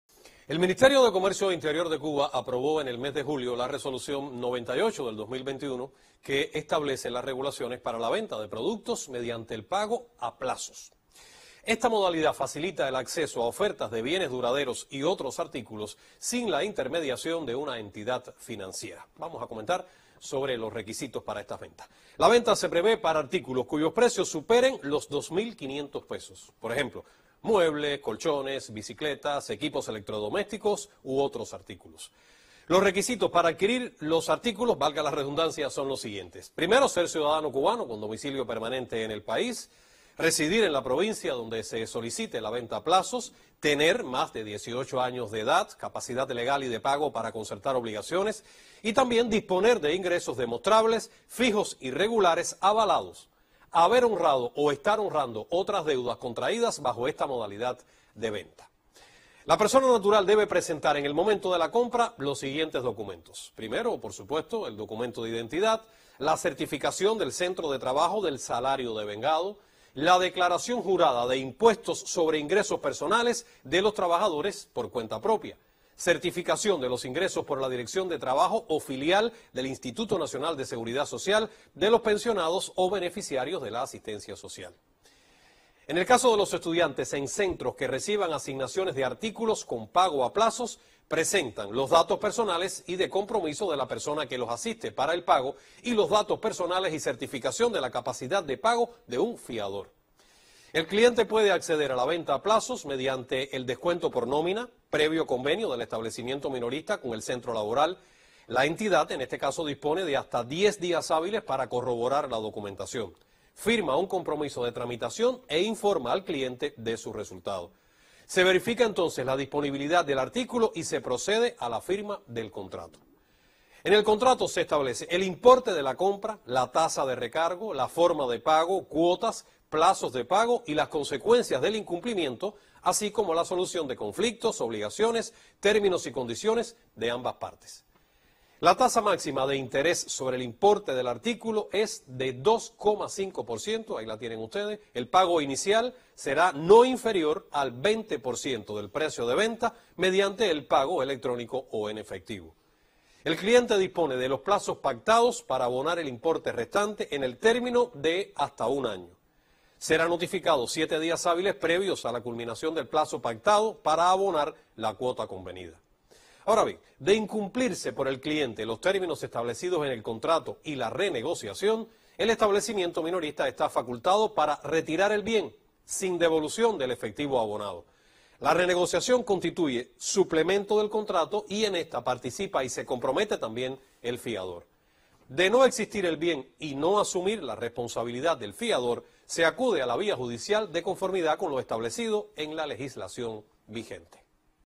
Compra a plazos en Cuba
El Ministerio de ComercioInterior de Cuba aprobó en el mes de Julio la Resolución 98/2021, que estable las regulaciones para la venta de productos mediante el PagoAPlazos. Esta modalidad facilita el acceso a ofertas de bienes duraderos y otros artículos, sin la intermediación de una entidad financiera. La venta se prevé para artículos cuyos precios superen los 2 500 pesos. Por ejemplo muebles, colchones, bicicletas, equipos electrodomésticos y otros artículos.
Los requisitos para adquirir los artículos son:
- Ser ciudadano cubano con domicilio permanente en el país,
- Residir en la provincia donde se solicite la venta a plazos,
- Tener más de 18 años de edad, capacidad legal y de pago para concertar obligaciones,
- Tener ingresos demostrables, fijos y regulares avalados, y
- Haber honrado o estar honrando otras deudas contraídas bajo esta modalidad de venta.
La persona natural debe presentar en el momento de la compra los siguientes documentos:
- Documentos de identidad,
- Certificación del centro de trabajo del salario devengado,
- Declaración jurada de impuestos sobre ingresos personales, de los trabajadores por cuenta propia,
- Certificación de los ingresos por la Dirección de Trabajo o filial del Instituto Nacional de Seguridad Social, de los pensionados o beneficiarios de la asistencia social,
- Los estudiantes en centros que reciban asignaciones de artículos, con pago a plazos, presentan los datos personales y compromiso de la persona que los asiste para el pago, y
- Datos personales y certificación de la capacidad de pago de un fiador.
El cliente puede acceder a la venta a plazos mediante el descuento por nómina, previo convenio del establecimiento minorista con el centro laboral.
La entidad dispone de hasta 10 días hábiles para corroborar la documentación. Firma un compromiso de tramitación e informa al cliente de su resultado. Se verifica la disponibilidad del artículo y se procede a la firma del contrato.
En el mismo se establecen: el importe de la compra, tasa de recargo, forma de pago, cuotas, plazos de pago, y las consecuencias del incumplimiento, así como la solución de conflictos, obligaciones, términos y condiciones de ambas partes.
La tasa máxima de interés sobre el importe del artículo es de 2,5 por ciento. El pago inicial será no inferior al 20 por ciento del precio de venta, mediante el pago electrónico o en efectivo. El cliente dispone de los plazos pactados para abonar el importe restante, en el término de hasta un año.
Será notificado siete días hábiles previos a la culminación del plazo pactado para abonar la cuota convenida.
De incumplirse por el cliente los términos establecidos en el contrato y la renegociación, el establecimiento minorista está facultado para retirar el bien, sin devolución del efectivo abonado.
La renegociación constituye suplemento del contrato, y en esta participa y se compromete también el fiador. De no existir el bien y no asumir la responsabilidad el fiador, se acude a la vía judicial de conformidad con lo establecido en la legislación vigente.